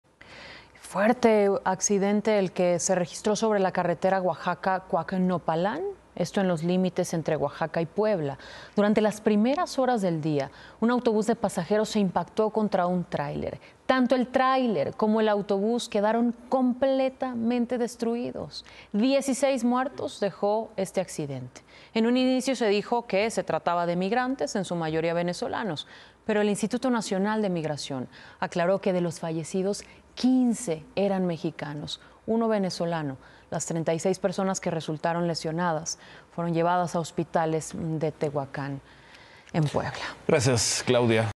Choque de autobús de pasajeros y tráiler deja 16 muertos en autopista Cuacnopalan-Oaxaca | Ciro
CiroEnImagen Un autobús de pasajeros y un tráiler se vieron involucrados en un choque sobre la autopista CuacnopalanOaxaca, dejando un saldo de 16 muertos y 36 personas lesionadas.